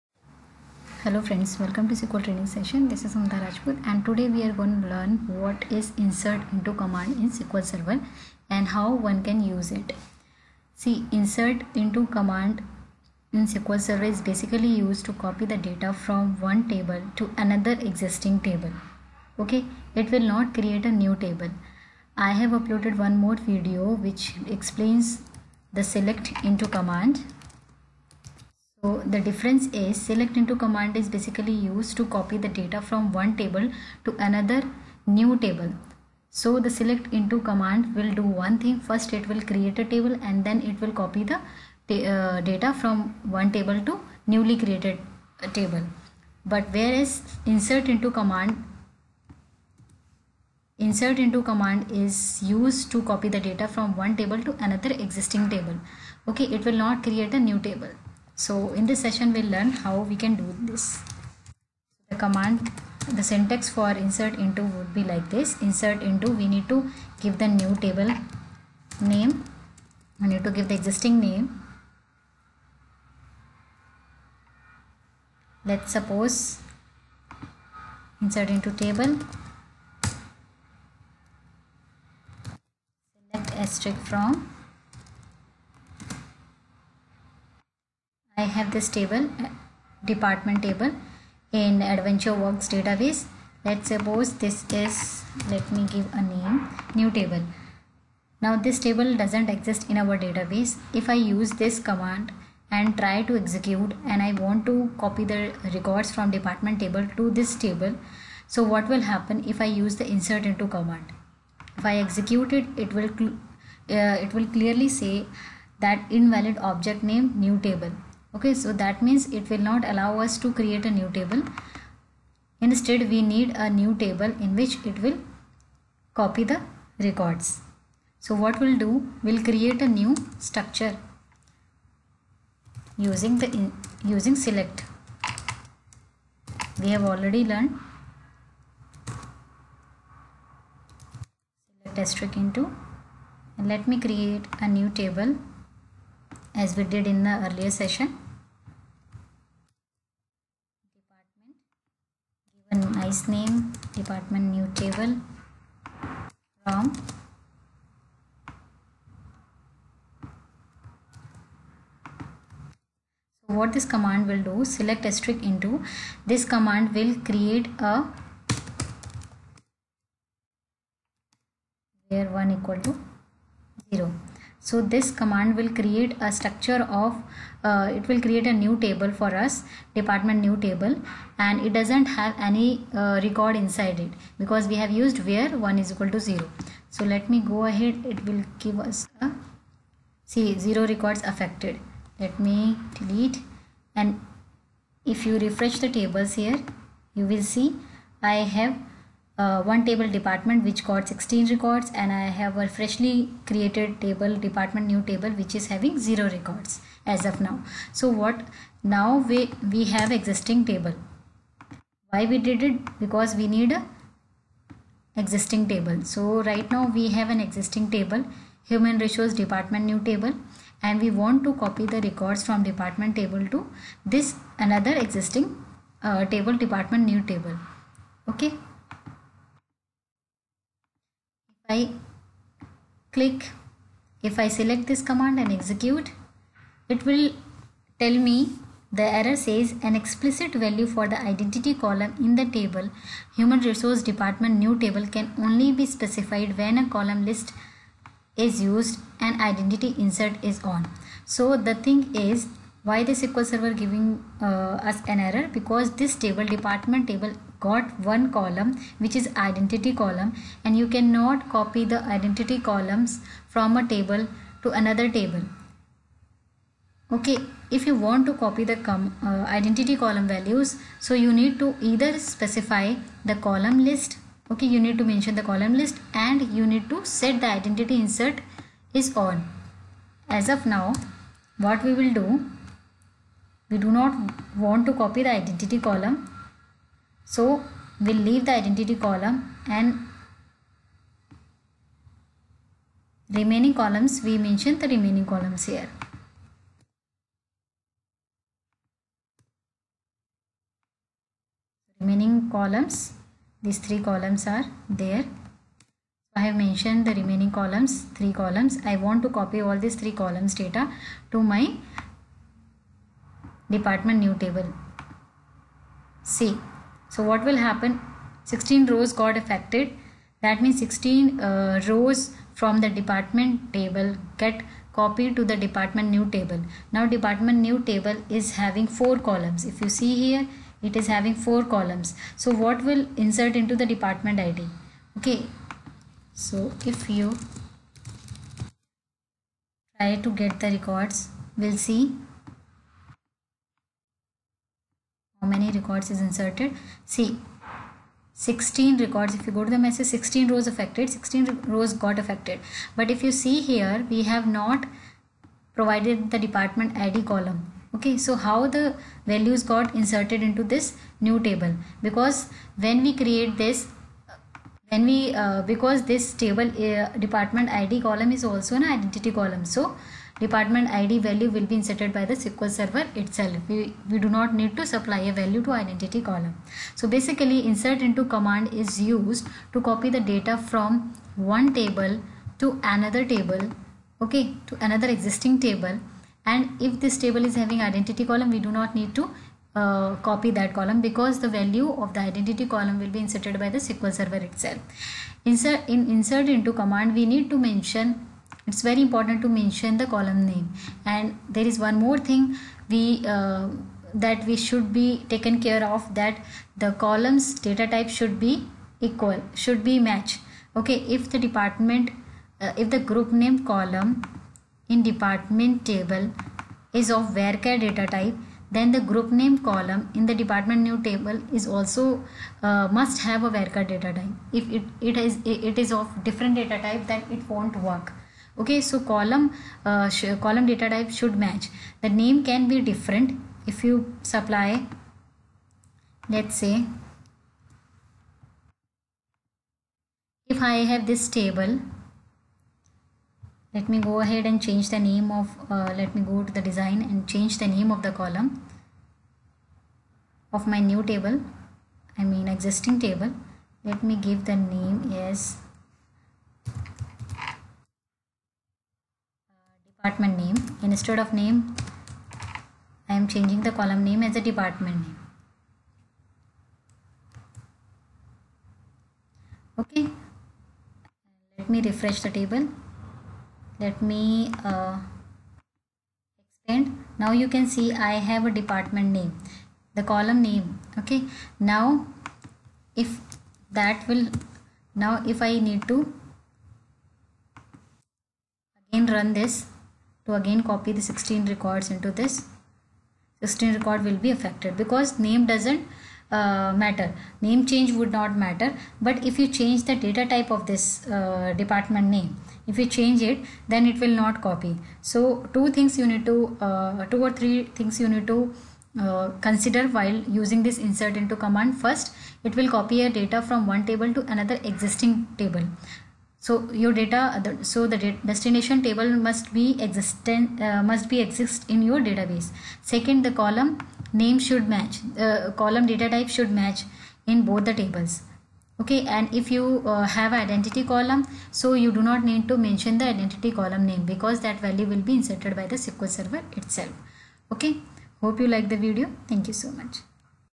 INSERT INTO SELECT in sql server | By SQL Training
This session will help you understand the following:
What is  INSERT INTO SELECT Command in sql server
syntax of INSERT INTO
Difference between INSERT INTO SELECT and SELECT INTO in sql server

By SQL Training Sessions
By SQL